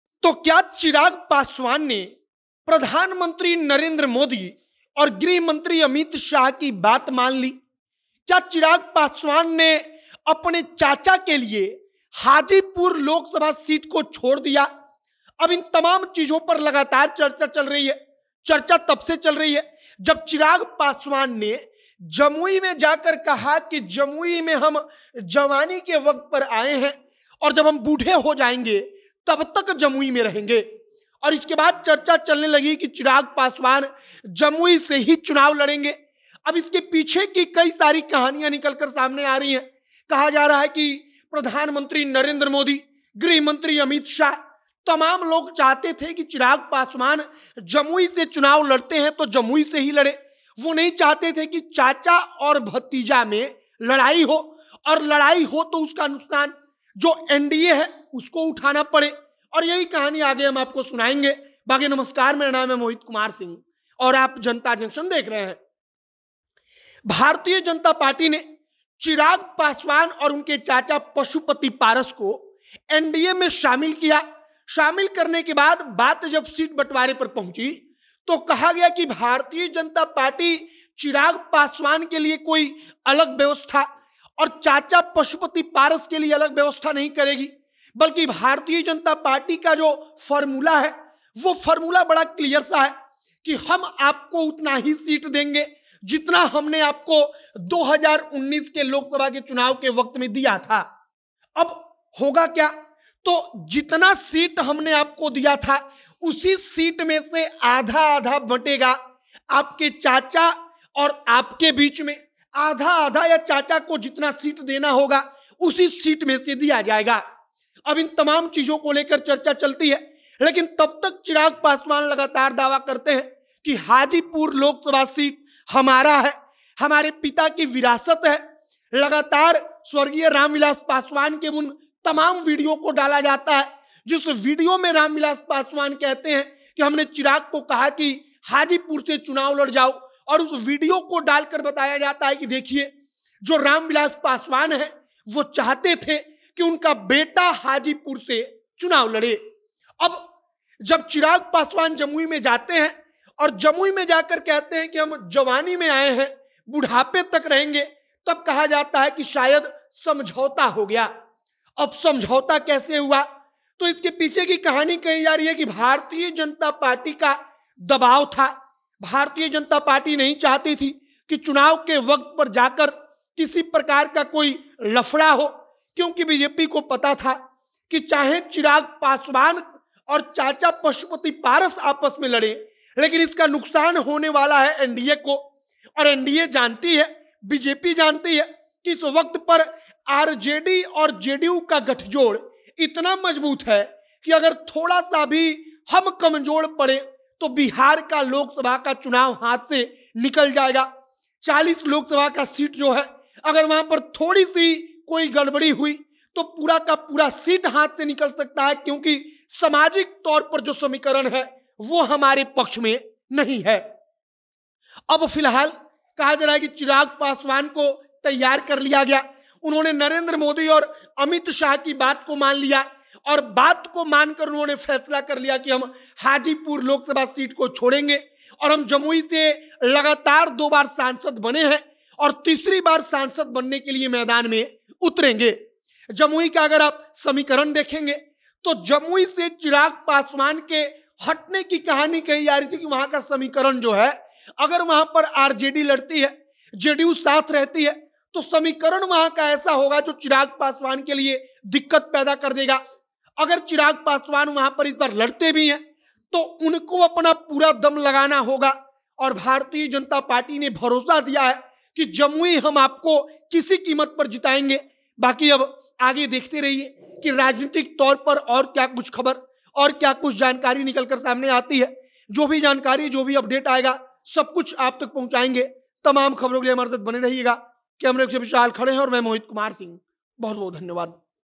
Chirag Paswan को BJP ने समझाया फिर उन्होंने Hajipur LokSabha Seat को छोड़ दिया, जानिए कहानी!
चिराग पासवान को भारतीय जनता पार्टी ने समझाया और फिर वह हाजीपुर लोकसभा सीट को छोड़ने का ऐलान कर देते हैं! जानिए चिराग पासवान ने जमुई से क्या वादा किया है?